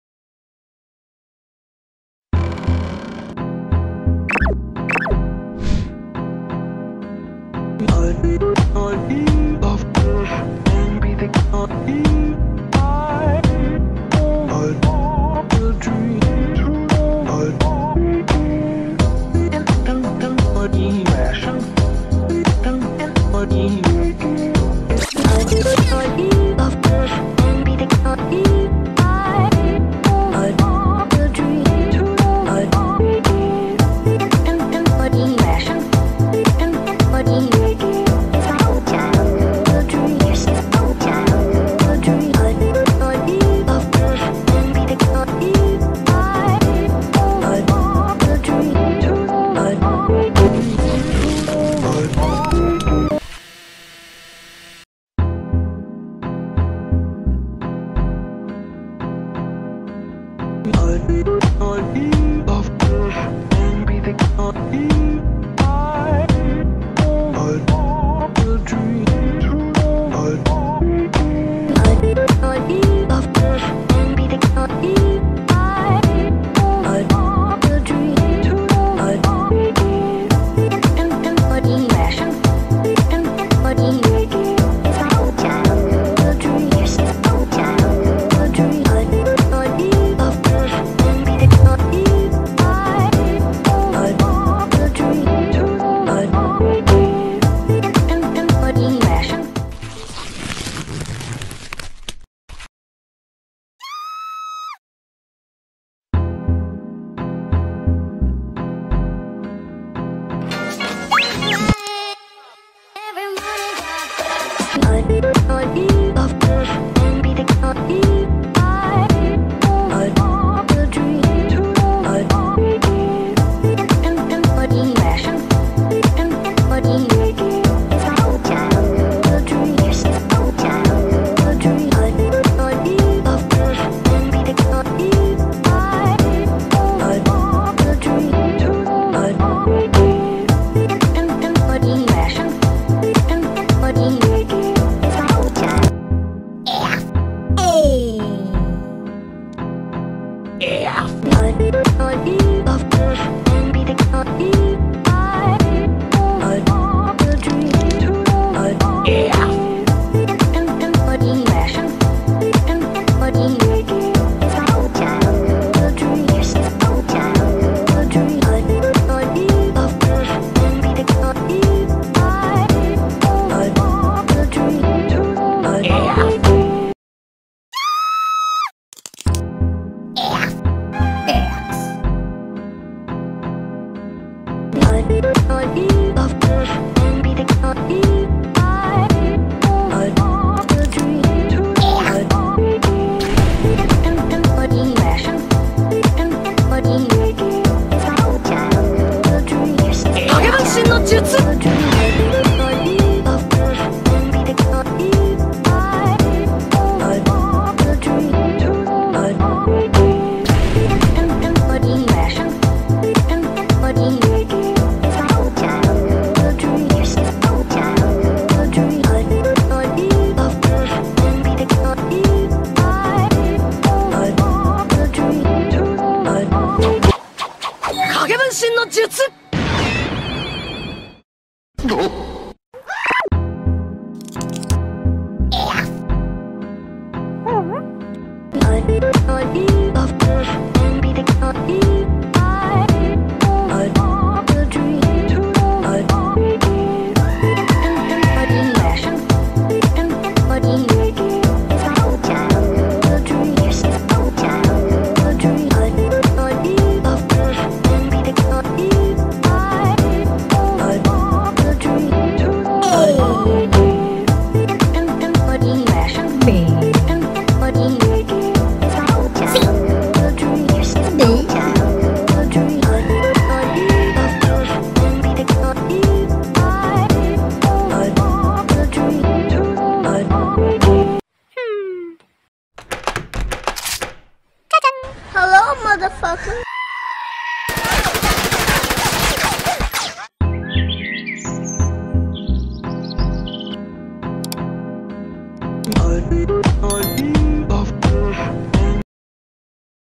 제로투 피하기 밈 모음집 - 레인보우프렌즈 / 알파벳로어 F ,X /Forget meme
안녕하세요~ 하츠입니다  어떤 캐릭터가 마음에드나요?
제로투 피하기 밈 모음집 - 레인보우프렌즈  / 알파벳로어 F ,X /Forget meme/ Rainbow Friends  Zero Two Dodging meme Complete Edition 2 (feat. alphabet lore F, X/ Rainbow Friends blue, red )

00:00 Green
00:55 red
01:50 Blue
02:25 alphabet lore A
03:00 alphabet lore X
03:55 alphabet lore F